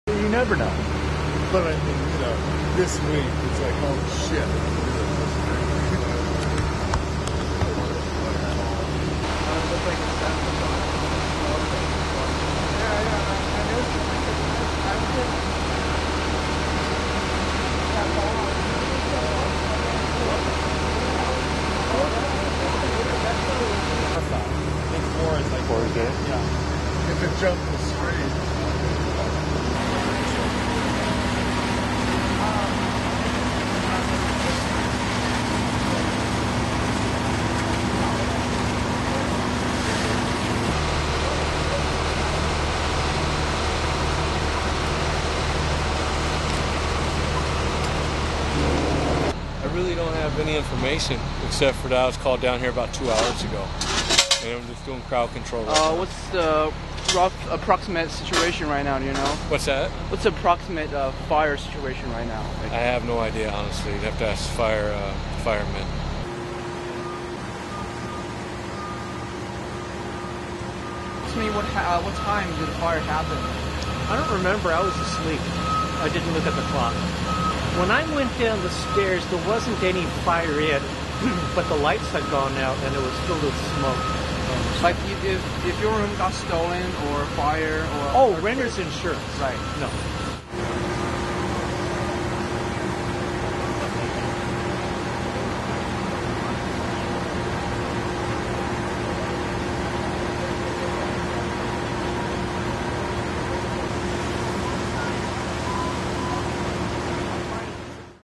Berkeley Fire. Haste and Telegraph --11/18/11
The fire started around 8:30 PM, Nov. 18th, at Haste St. and Telegraph Ave. Berkeley, California